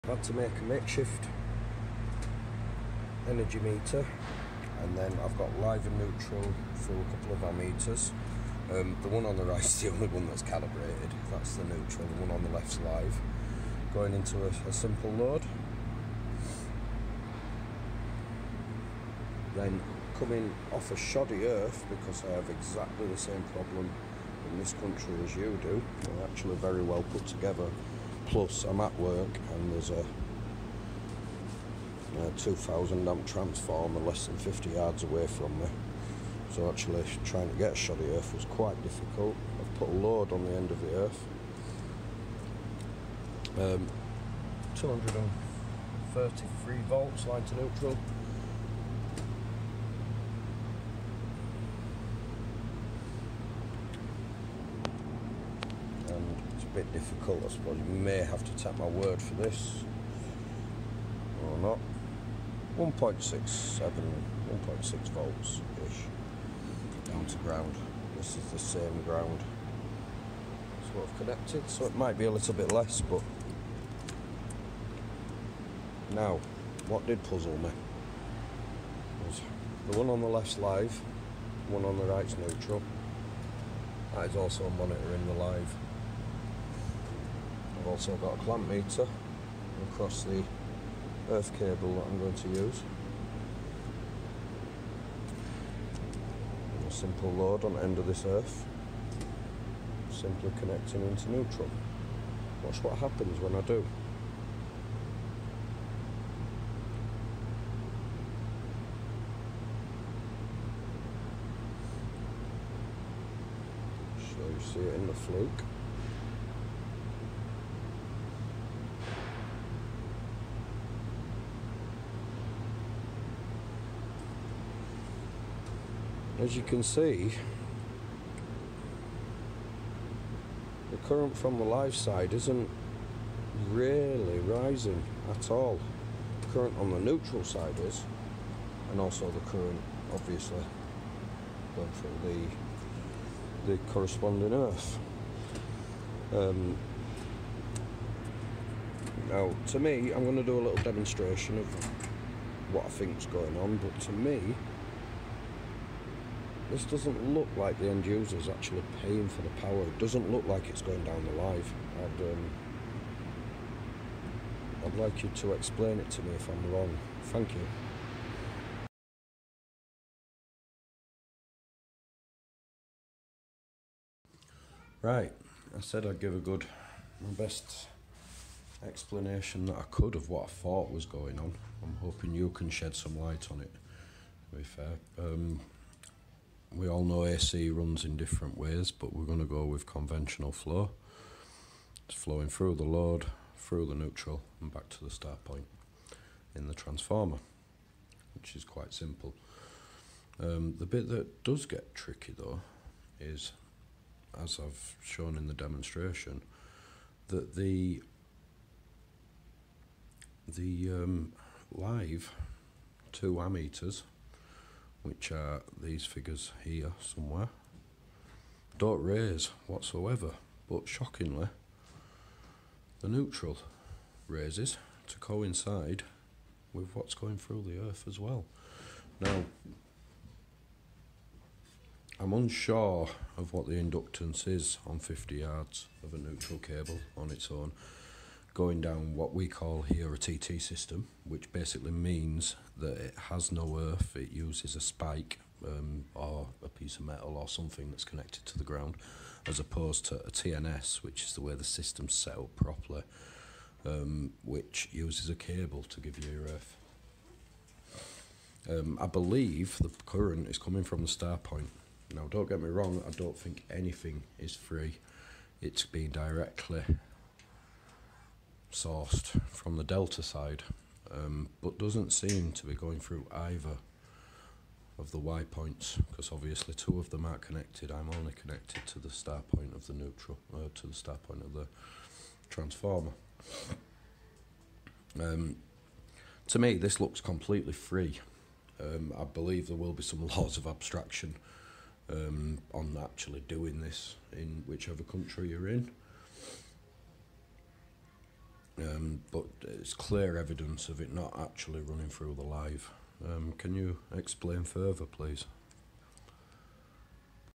Response to electroBOOM 3 million subs. There is free energy in these cables.
I posted a version of this a while back but didn't get a explanation, so iv decided to re upload it with the name in the title. I don't want to get into a war but in this case I don't think you're correct. The induced current doesn't register on the live side of a single phase supply. It must be coming from the delta of the transformer however doesn't register on the y side of the installation you're using.

Original video of electroBOOM 3 million subs